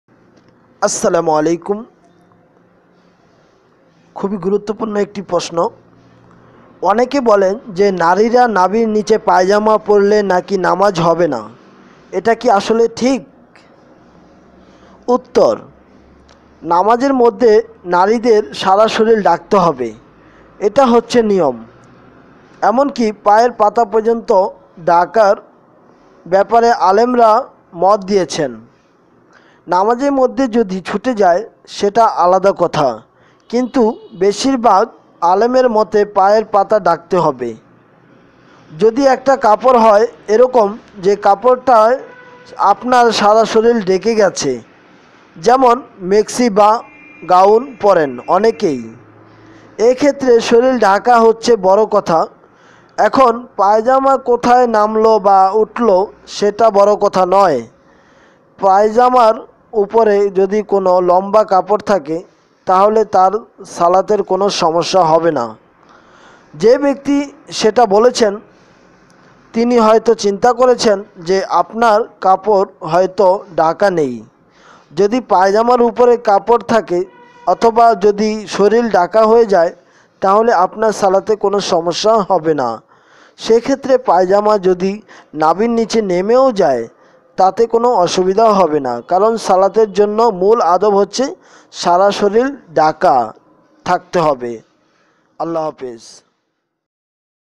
নারীরা নাভির নিচে পায়জামা পরলে কি নামাজ হবে ?
প্রশ্ন : অনেকে বলেন যে নারীরা নাভির নিচে পায়জামা পরলে নাকি নামাজ হবে না। এটা কি আসলে ঠিক?
উত্তর : নামাজের মধ্যে নারীদের সারা শরীর ঢাকতে হবে, এটা হচ্ছে নিয়ম। এমনকি পায়ের পাতা পর্যন্ত ঢাকার ব্যাপারে আলেমরা মত দিয়েছেন। নামাজের মধ্যে যদি ছুটে যায়, সেটা আলাদা কথা। কিন্তু বেশির ভাগ আলেমের মতে পায়ের পাতা ঢাকতে হবে। যদি একটা কাপড় হয় এ রকম, যে কাপড়টায় আপনার সারা শরীর ঢেকে গেছে, যেমন : ম্যাক্সি বা গাউন পরেন অনেকেই। এ ক্ষেত্রে শরীর ঢাকা হচ্ছে বড় কথা। এখন পায়জামা কোথায় নামল বা উঠল, সেটা বড় কথা নয়। পায়জামার ওপরে যদি কোনো লম্বা কাপড় থাকে, তাহলে তাঁর সালাতের কোনো সমস্যা হবে না।
যে ব্যক্তি সেটা বলেছেন, তিনি হয়তো চিন্তা করেছেন যে আপনার কাপড় হয়তো ঢাকা নেই। যদি পায়জামার ওপরে কাপড় থাকে অথবা যদি শরীর ঢাকা হয়ে যায়, তাহলে আপনার সালাতে কোনো সমস্যা হবে না। সে ক্ষেত্রে পায়জামা যদি নাভির নিচে নেমেও যায়, তাতে কোনো অসুবিধা হবে না। কারণ সালাতের জন্য মূল আদব হচ্ছে সারা শরীর ঢাকা থাকতে হবে।